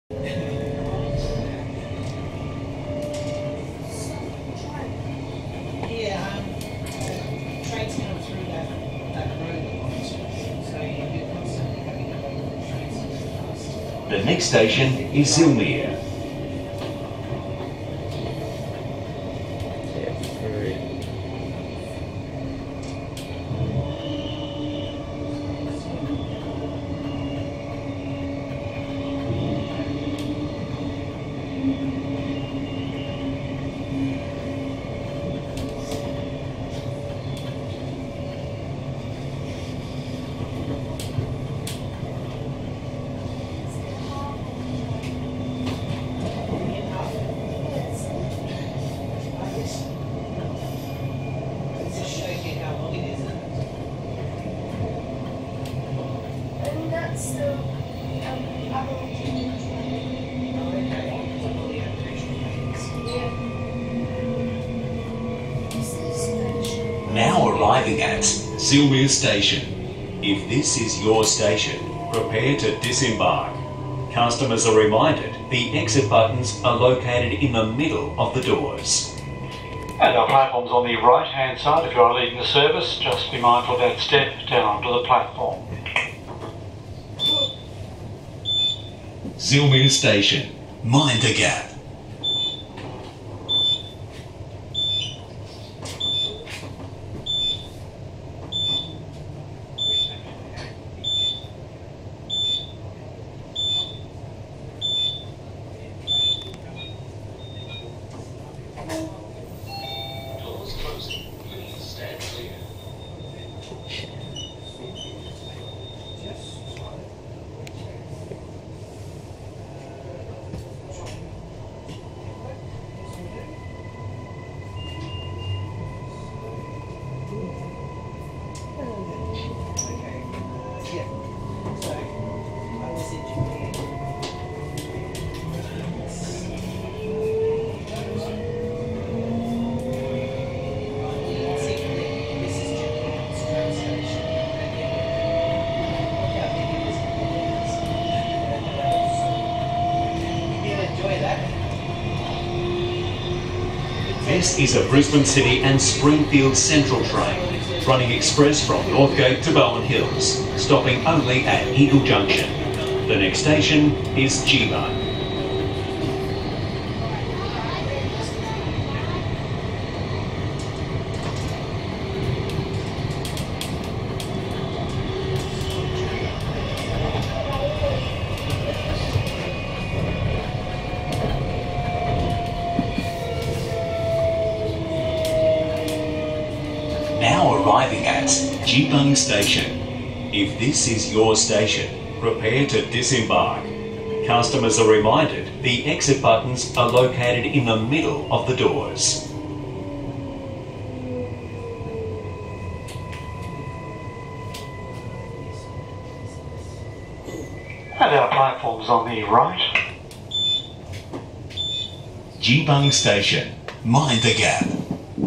Queensland Rail New Generation Rollingstock (NGR 759): Zilmere to Geebung (Springfield train)
Queensland Rail's New Generation Rollingstock (also known as the NGR) were built by Bombardier Transportation in Savii and were first delivered in 2016. They are the successor to the electric multiple units and InterCity Express rollingstock and currently newest type of rollingstock in revenue service. Unlike all previous rollingstock, NGRs are semi-permanently coupled six-car sets, feature one toilet per six-car movement and do not have exposed couplers.
It's now my turn to film the guard who makes Queensland Rail's best guard announcements.

Set number: NGR 759
Car number: 3759
Stations: Bald Hills, Strathpine, Bald Hills
Destination: Redcliffe Peninsula

Camera used: Nikon Coolpix A900
Resolution: 1920x1080p
Video 3 of 6 from June 26, 2021.